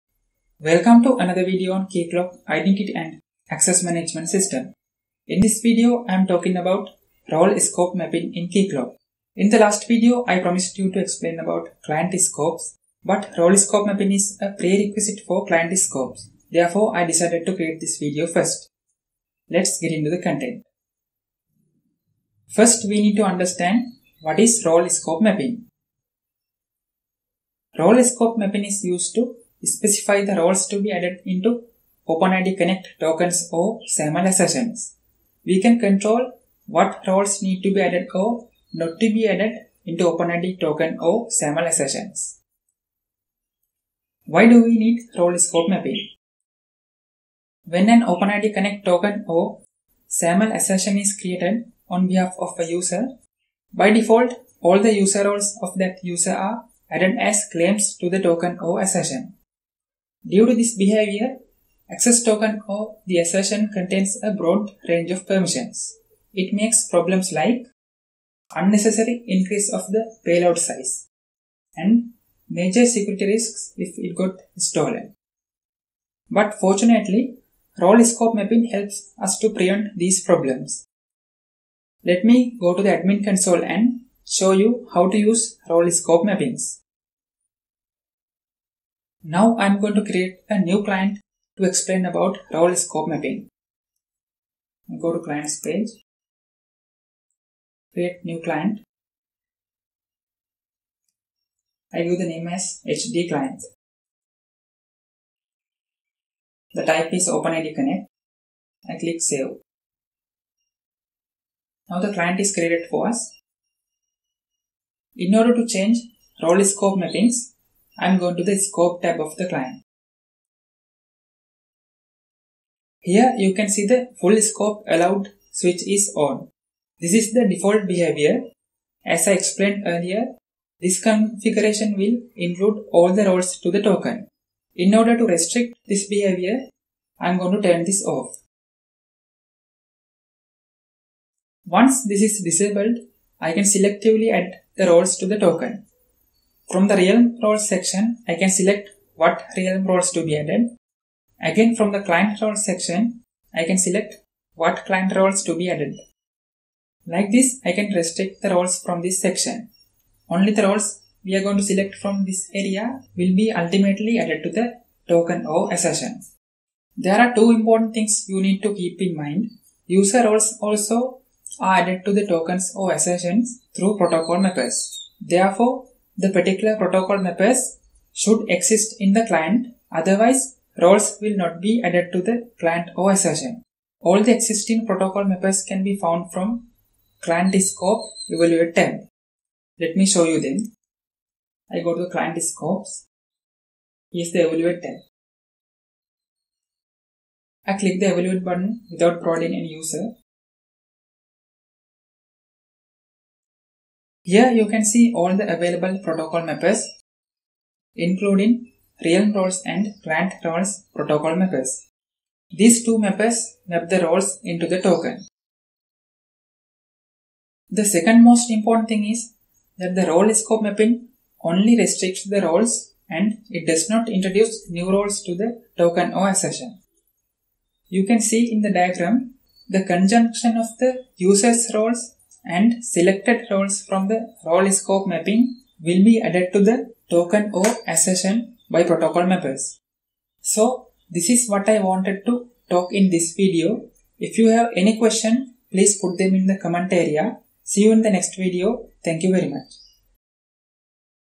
Keycloak Tutorial #13 - Role Scope Mapping
Explain about how to restrict the inclusion of user roles into the OIDC tokens or SAML assertions.

This is the 13th video (Role Scope Mapping) of a video series on Keycloak identity & access management system. This video shows how to restrict the inclusion of user roles into the OIDC tokens or SAML assertions.

All the user roles are included to the tokens or assertions by default.
This behavior causes problems such as increased token/assertion size, and security risks. Role scope mapping helps to mitigate this issue by allowing to add only the required roles (realm roles and client roles).